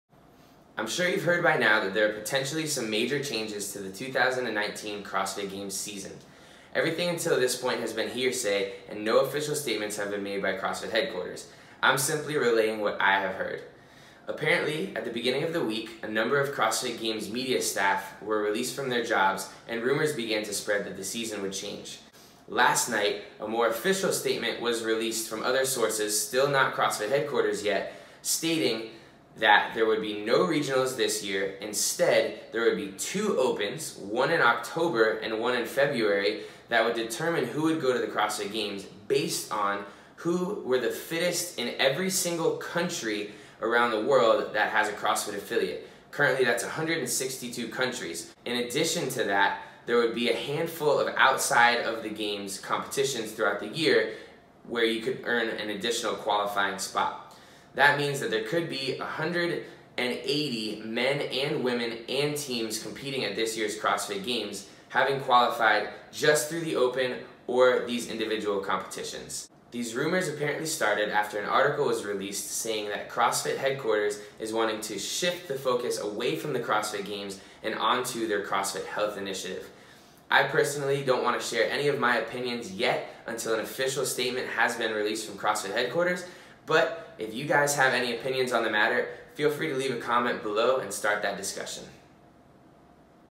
Potentially MAJOR changes to 2019 CrossFit Games season
Find out which rumors are circulating about some of the changes that we may see take effect this season. No official statements have been made yet by CrossFit HQ.

I’m curious what your thoughts are on the newly proposed structure. Will it work? Does it make sense? Would you miss regionals? Would this structure truly have the “fittest on earth” fighting for that title at the Games?